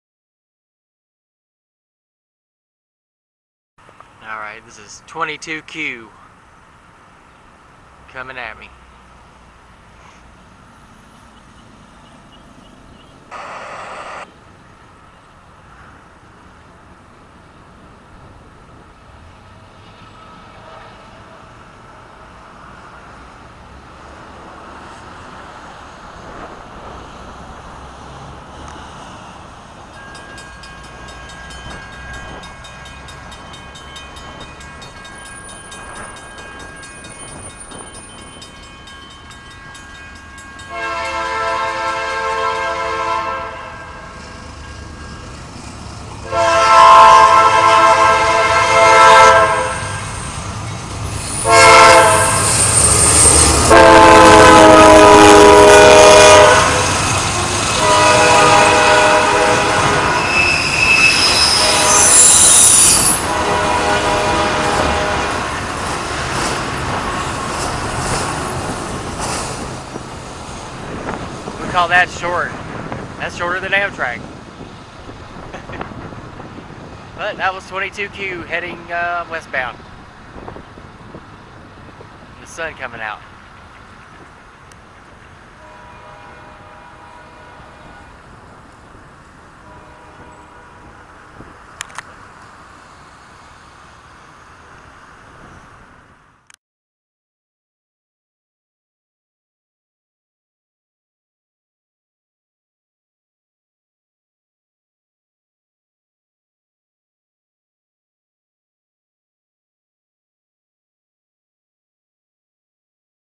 Norfolk Southern 22Q With Cool Crew & Horn! Austell,Ga 05-21-2009©
Norfolk Southern NS 22Q Heading Westbound in Austell,Ga with Cool Crew!